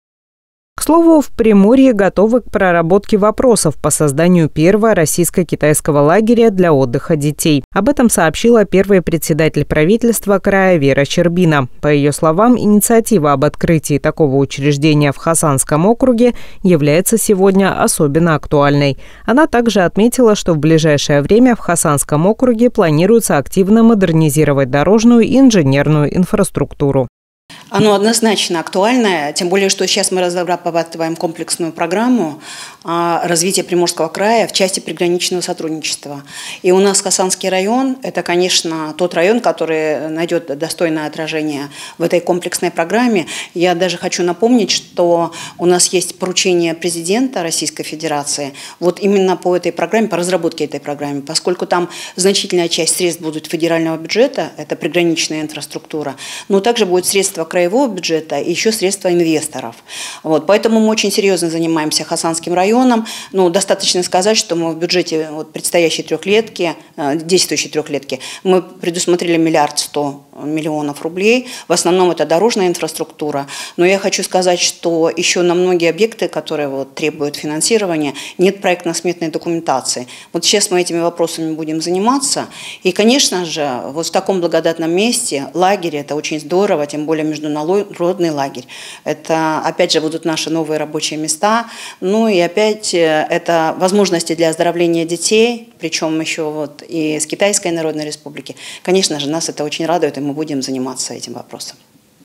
Приморье готово заниматься вопросами по созданию лагеря отдыха для детей России и КНР.
В Приморье готовы к проработке вопросов по созданию первого российско-китайского лагеря для отдыха детей. Об этом сообщила первый председатель правительства края Вера Щербина.  По ее словам, инициатива об открытии такого учреждения в Хасанском округе является сегодня особенно актуальной. Она также отметила, что в ближайшее время в Хасанском округе планируется активно модернизировать дорожную и инженерную инфраструктуру.